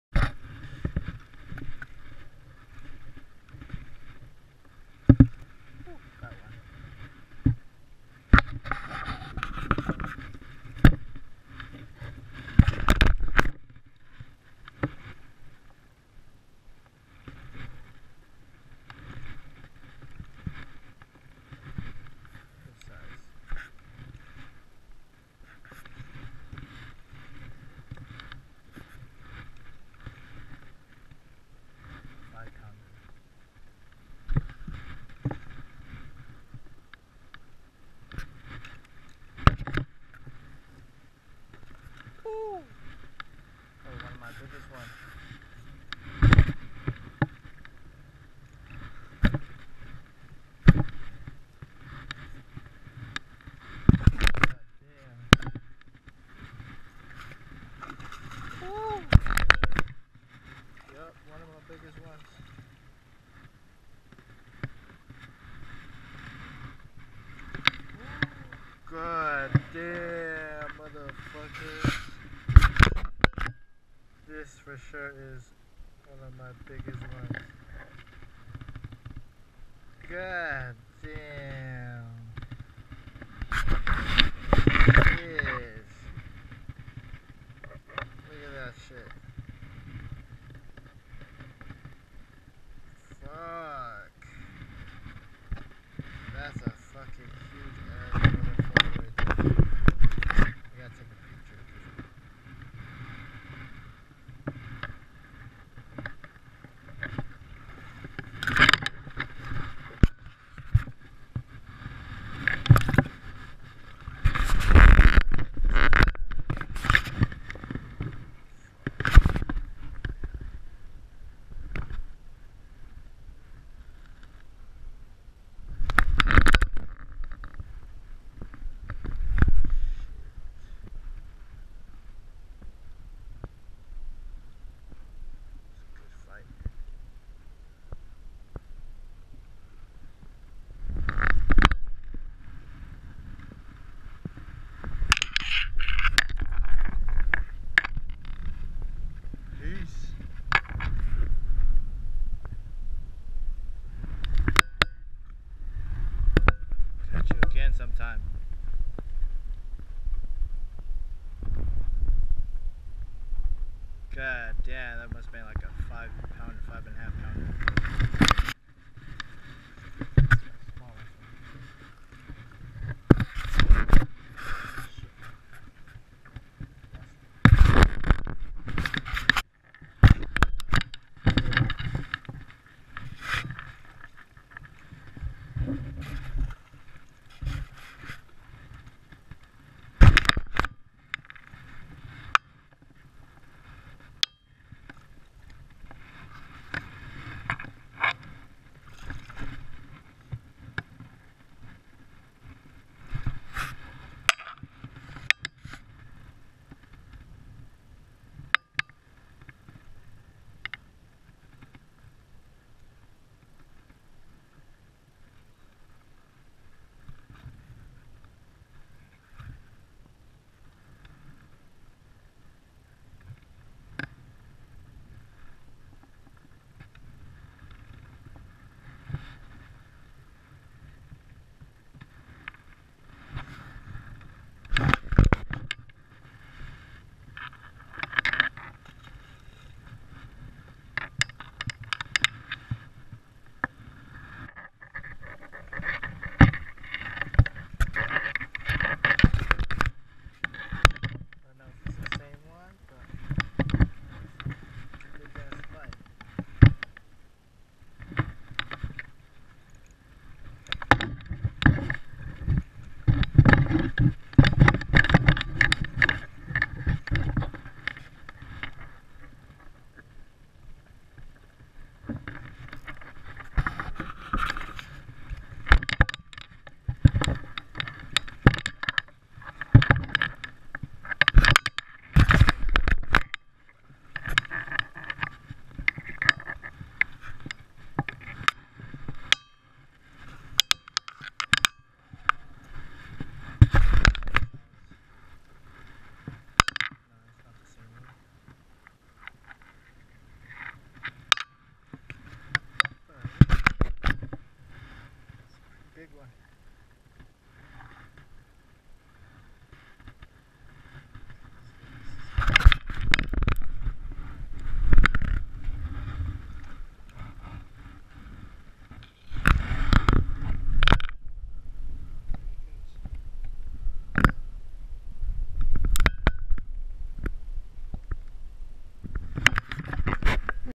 Jeys Big Fish Day
Was a little slow but had a good day fishing bass